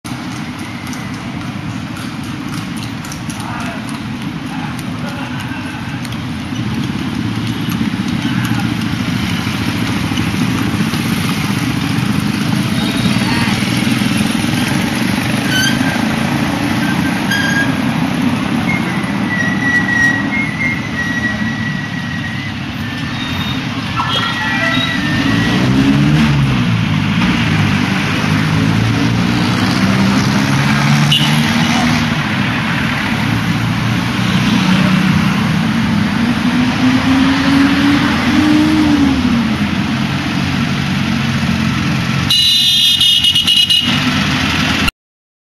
Horse-drawn carriage and Cathedral in Manila
Horse-drawn carriage and Cathedral in Manila seen nearby the China Town.